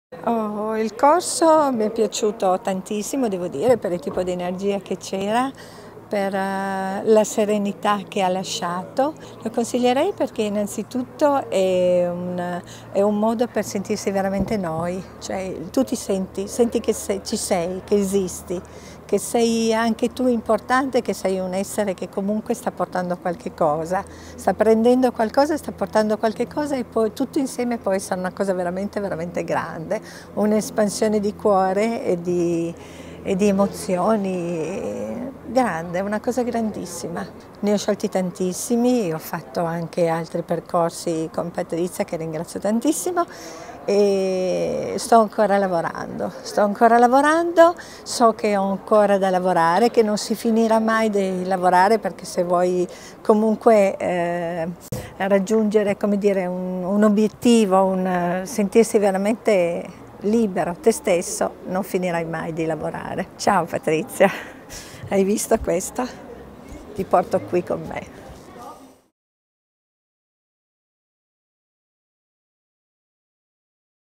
DCL Testimonianza di Loredana Zucca
"Senti che ci sei, che esisti, che sei anche tu importante e tutti insieme può essere una cosa grandissima"

La testimonianza di Loredana Zucca al Divine Connection Live di Reggio Emilia 2019